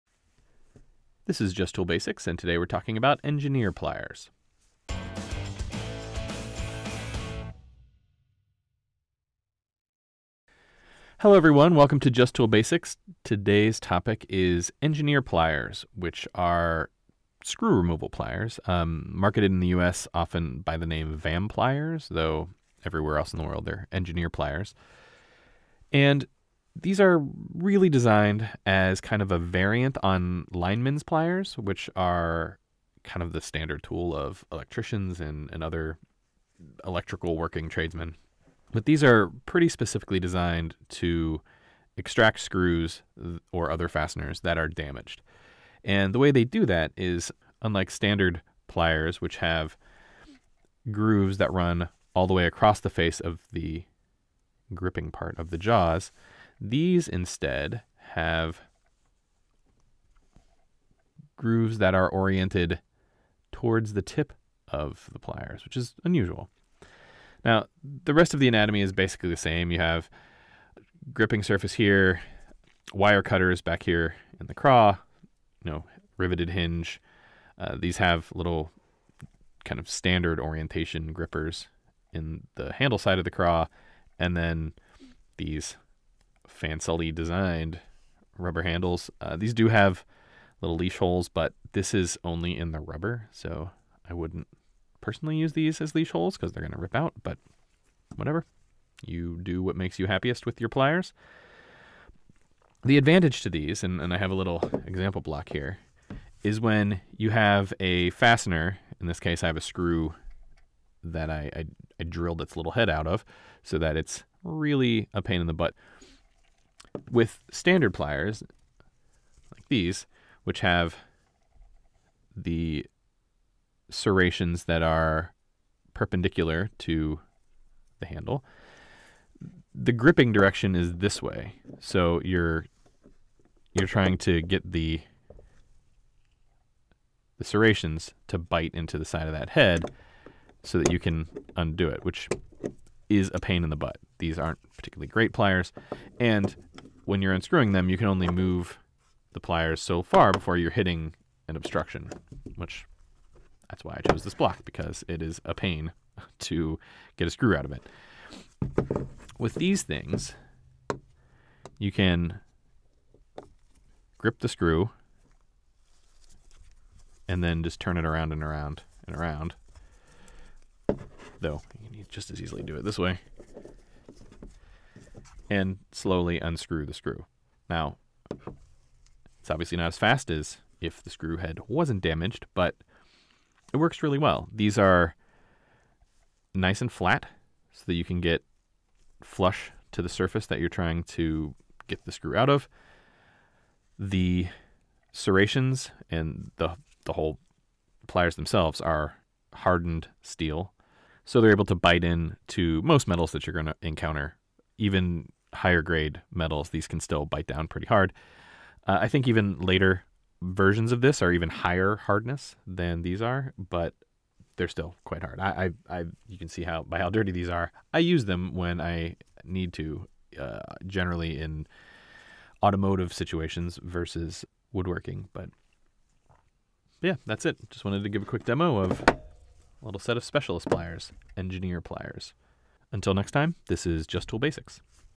Engineer Pliers / Vampliers Basics
The basics of engineer pliers (a.k.a. "Vampliers")

If you've ever struggled with pliers to remove a messed up screw, these specialized pliers are the answer!

Vampliers Pro (shown)

Engineer pz-59 pliers (exact same as the ones shown in the video except green handles and a bit cheaper)

Vampliers Brute (newer, fancier model than one shown in the video)